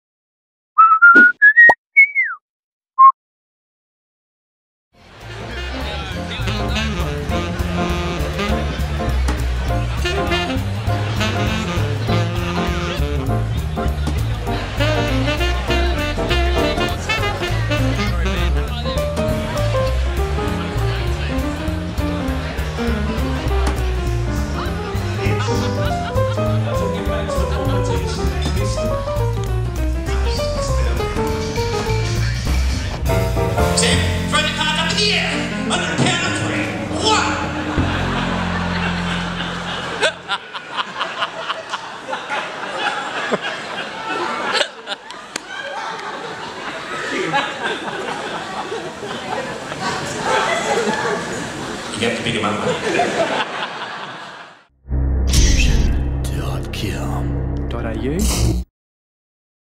Fusion at the AADC Awards
Fusion had a magic night at the 34th AADC Awards where we took home 5 Finalists and 3 Awards. Held at the Wine Centre in Adelaide, 7 March 2011 we were also entertained by comedian magician Matt Hollywood.